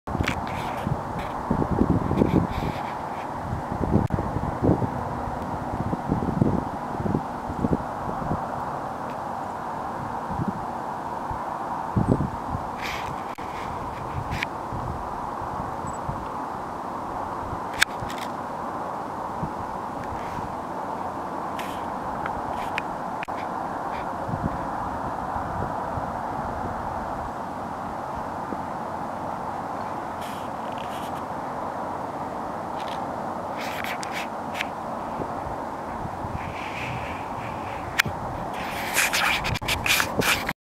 Chilliwack from Chilliwack Mountain
Chilliwack, B.C. veiwed from Chilliwack Mountain at dusk.